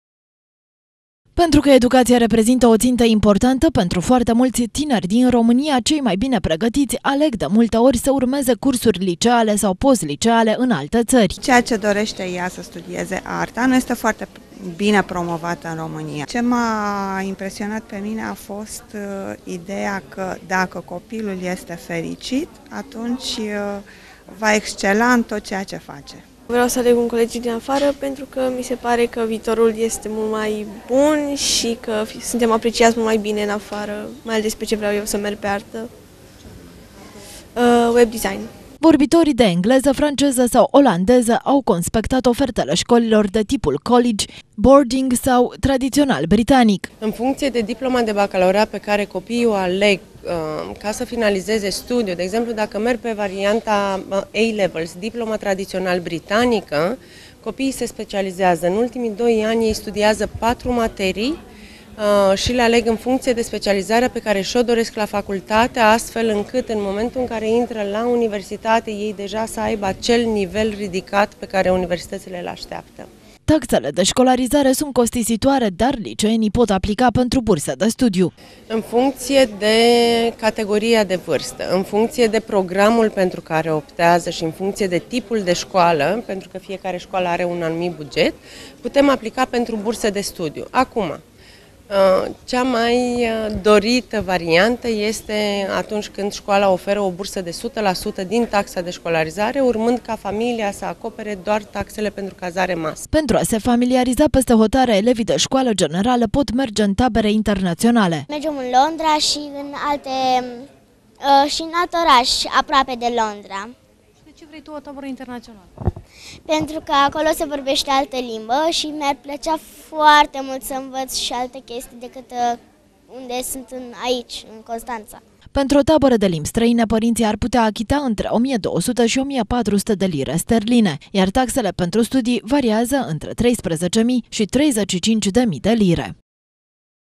Ştire Etv - Studiile peste hotare, dorinţa liceenilor
Zeci de tineri au aflat ce oportunităţi au pentru a învăţa la un liceu de peste hotare şi la ce tabere internaţionale pot să participe. Tinerii au primit răspunsuri la toate întrebările, de la consultanţi străini, în cadrul unui târg educaţional.